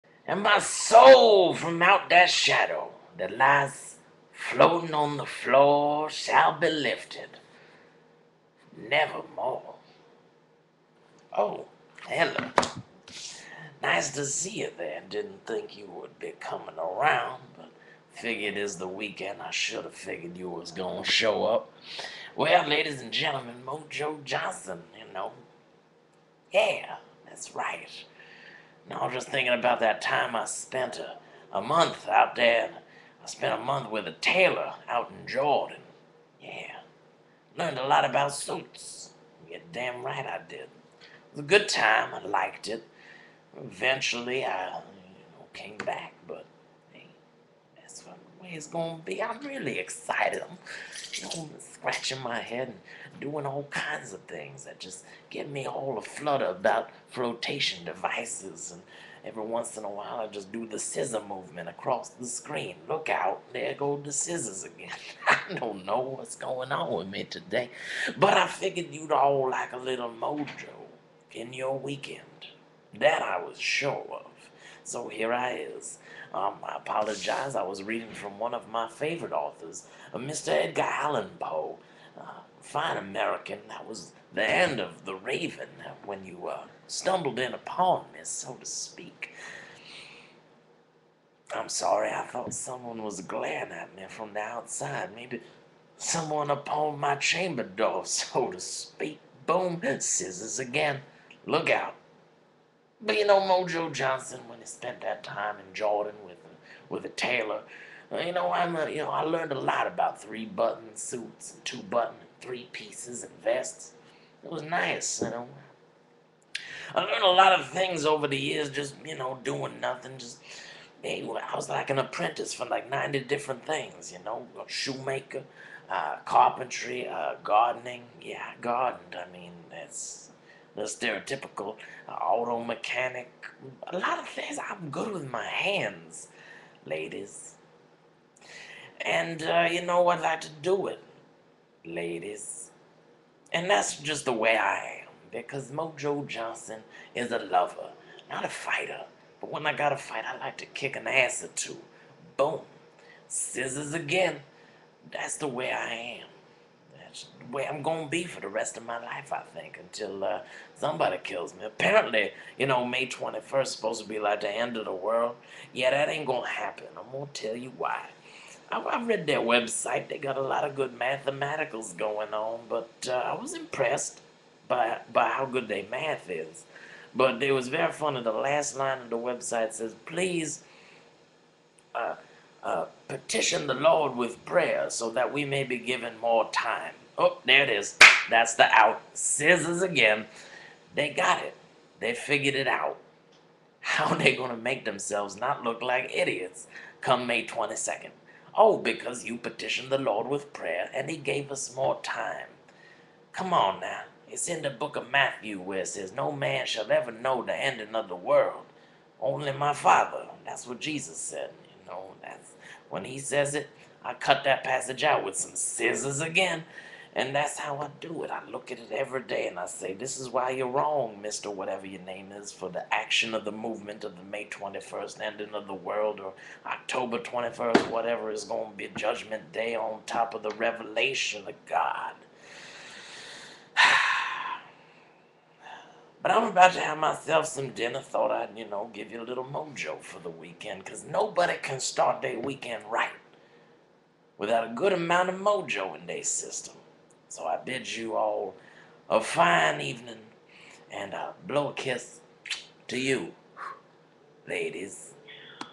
Mojo Johnson: May 21st is a Falsity
Mojo doesn't believe the world will be ending any time soon.